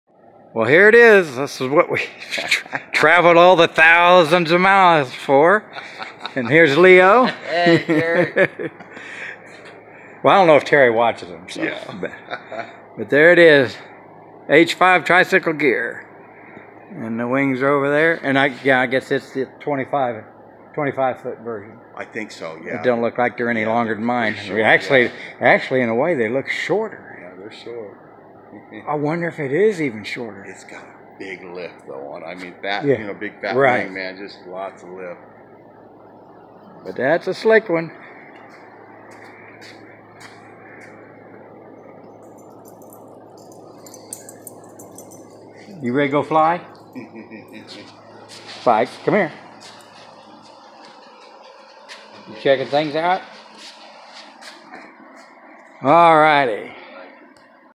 MADE IT!!!!!Here It is H5
Made it to Navada to get the H5. Sharp looking